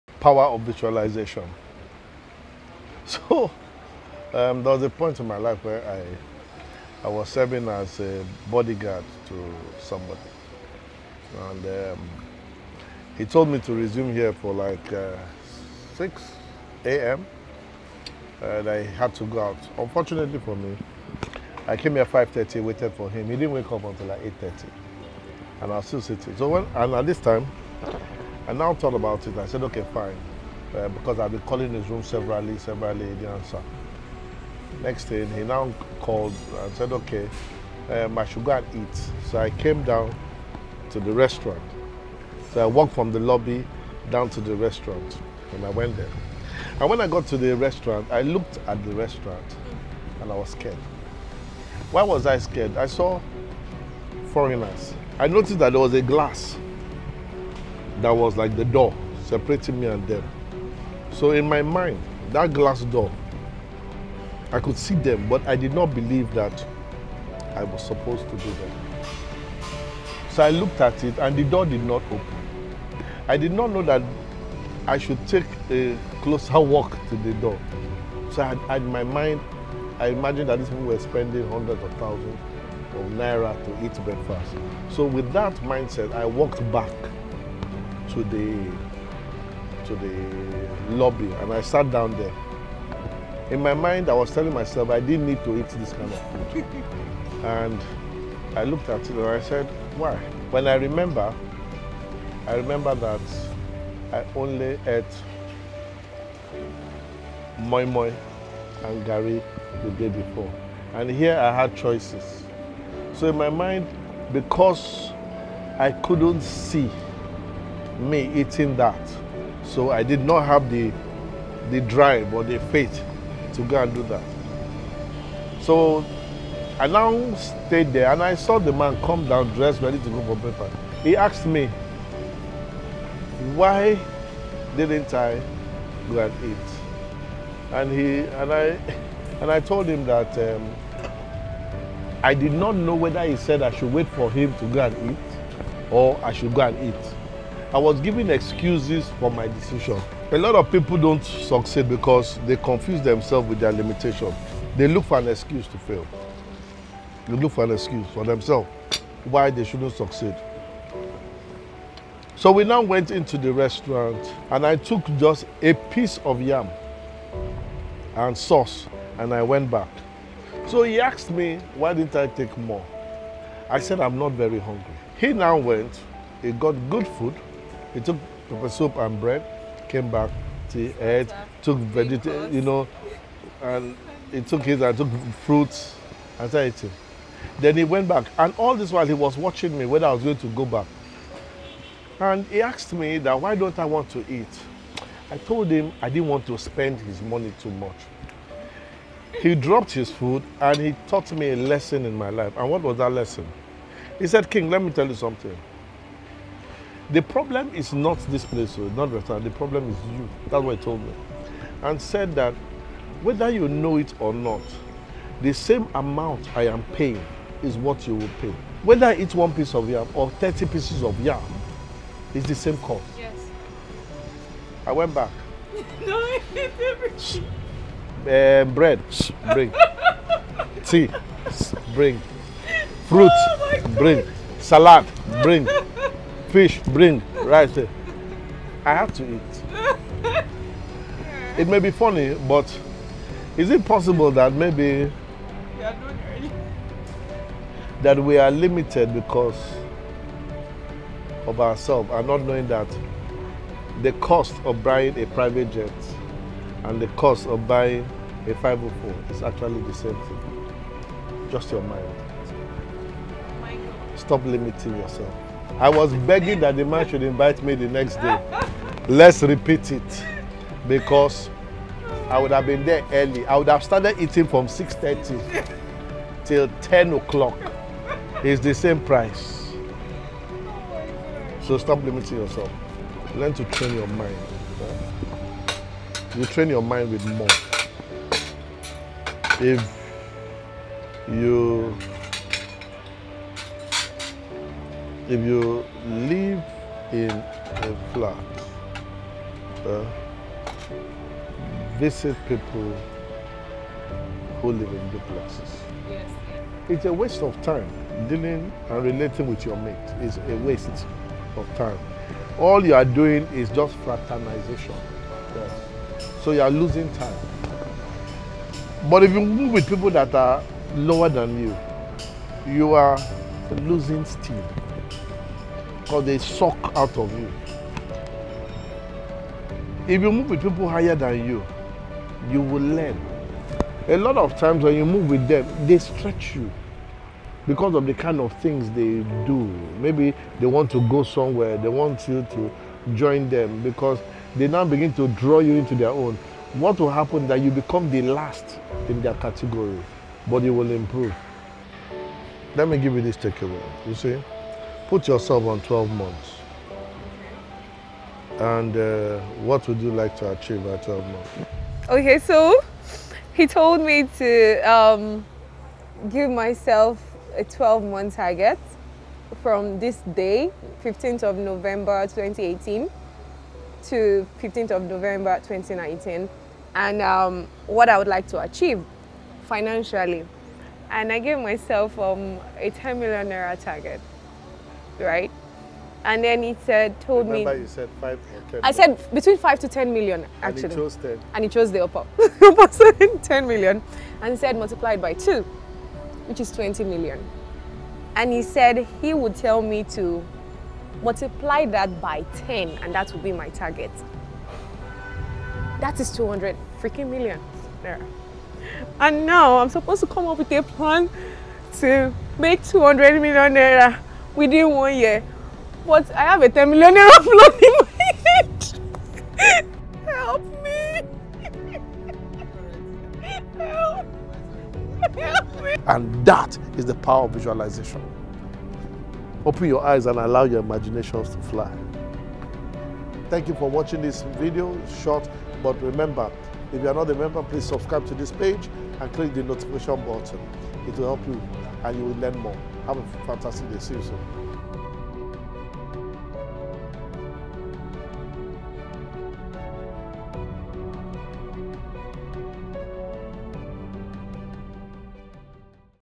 The power of Visualization | Coaching Over Lunch
We are not limited by our location, our education but more by our power to imagine (THE POWER OF VISUALIZATION)..

The power of seeing the vision of tomorrow (not the one they do in prayer houses). It's the ability to look at tomorrow though your eyes today.

Put up a VISION BOARD and allow your imagination to fly